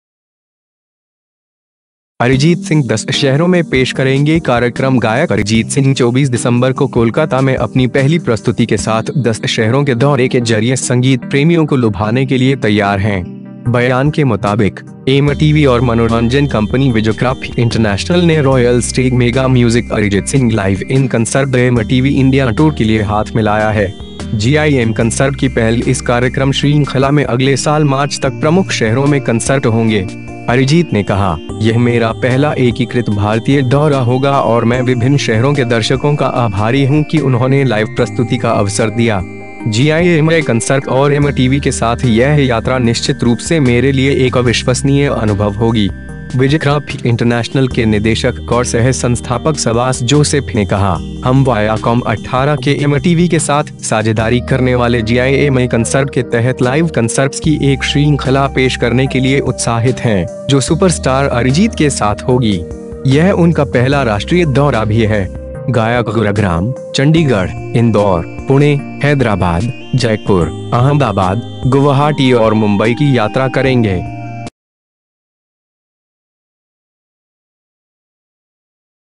अरिजीत सिंह 10 शहरों में पेश करेंगे कार्यक्रम
गायक अरिजीत सिंह 24 दिसंबर को कोलकाता में अपनी पहली प्रस्तुति के साथ 10 शहरों के दौरे के जरिए संगीत प्रेमियों को लुभाने के लिए तैयार हैं। बयान के मुताबिक, एमटीवी और मनोरंजन कंपनी विजक्राफ्ट इंटरनेशनल ने 'रॉयल स्टेग मेगा म्यूजिक अरिजित सिंह लाइव इन कन्सर्ट- द एमटीवी इंडिया टूर' के लिए हाथ मिलाया है।

जीआईएम कन्सर्ट की पहल इस कार्यक्रम श्रृंखला में अगले साल मार्च तक प्रमुख शहरों में कन्सर्ट होंगे।

अरिजीत ने कहा, "यह मेरा पहला एकीकृत भारतीय दौरा होगा और मैं विभिन्न शहरों के दर्शकों का आभारी हूं कि उन्होंने लाइव प्रस्तुति का अवसर दिया। जीआईएमए कंसर्ट और एमटीवी के साथ यह यात्रा निश्चित रूप से मेरे लिए एक अविश्वसनीय अनुभव होगी।"

विजक्राफ्ट इंटरनेशनल के निदेशक और सह-संस्थापक सब्बास जोसेफ ने कहा, "हम वायाकॉम 18 के एमटीवी के साथ साझेदारी करने वाले जीआईएमए कन्सर्ट के तहत लाइव कन्सर्ट्स की एक श्रृंखला पेश करने के लिए उत्साहित हैं, जो सुपरस्टार अरिजीत के साथ होगी। यह उनका पहला राष्ट्रीय दौरा भी है।"

गायक गुरग्राम, चंडीगढ़, इंदौर, पुणे, हैदराबाद, जयपुर, अहमदाबाद, गुवाहाटी और मुंबई की यात्रा करेंगे।